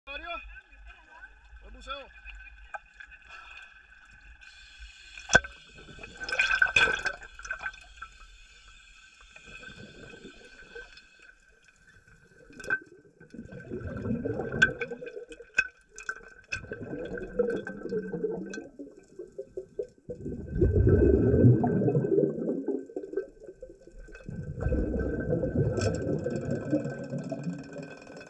RAW Footage - Using the BE360 Dome Diving
A preview clip from the GoPro Fusion using the BE360 Dome

-Up to 30mt-98ft deep
-Easy to use and transport
-Small footprit
-All of this with almost perfect stich line

For RAW footage and more updates